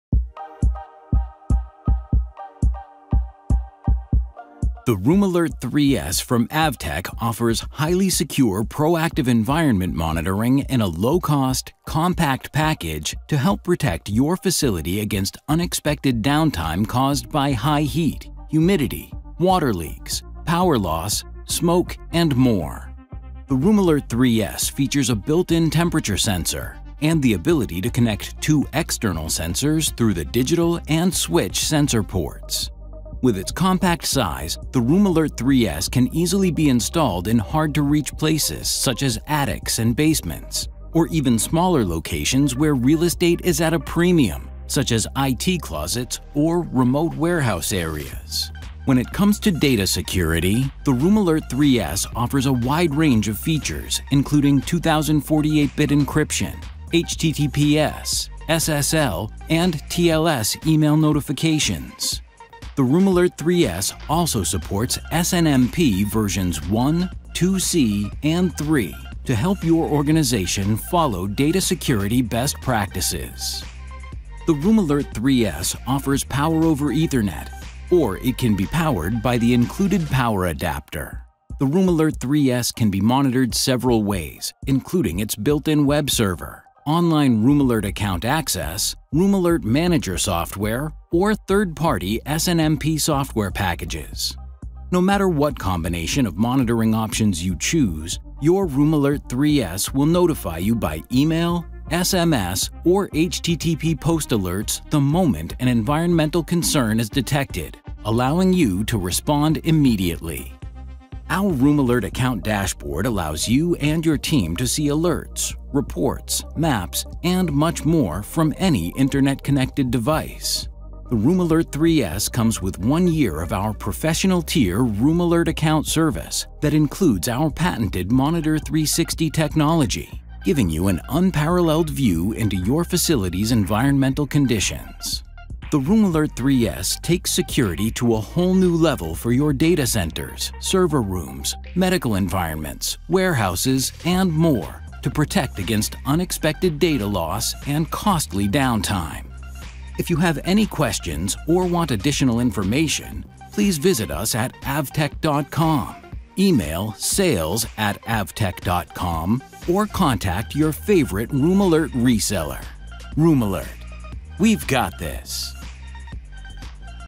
The Room Alert 3S – AVTECH's Most Economical & Compact Highly Secure Monitor
The Room Alert 3S is the ideal entry-level environment monitoring solution for users who require data privacy and security features. The Room Alert 3S  is a light-weight monitor that can fit easily in telecom or wiring closets, server rack cabinets or other small spaces.